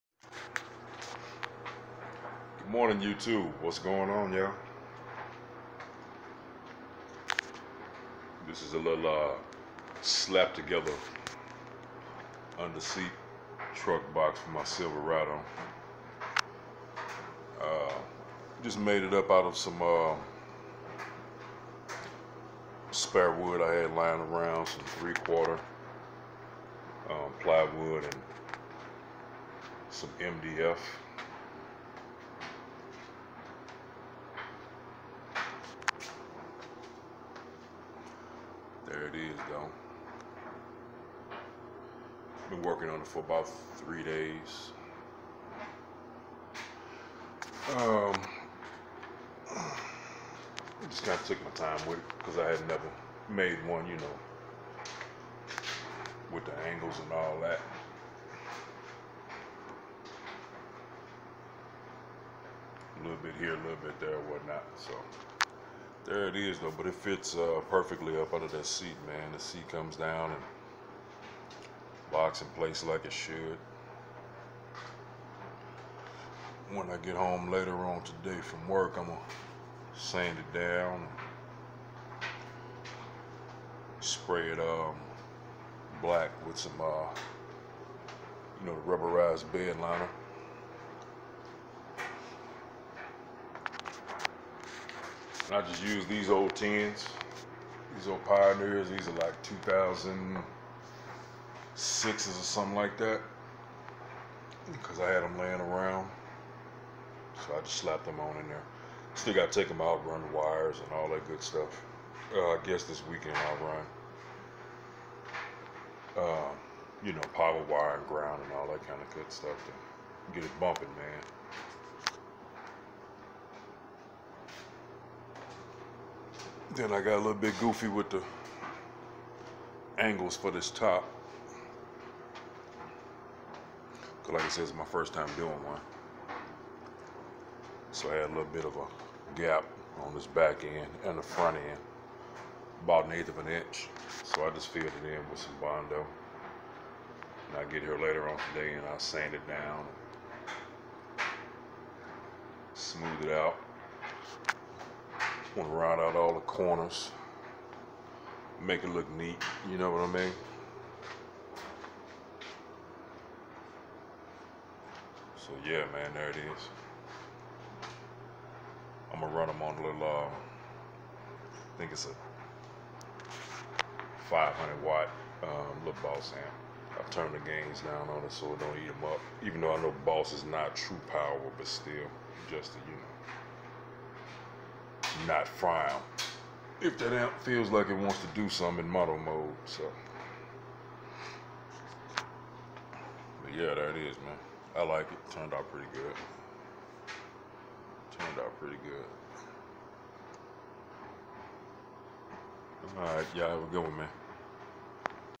Silverado Dual10"Under Seat Sub Box.
Dual 10" sub enclosure I slapped together from 3/4"  plywood and 1" MDF. turned out pretty descent.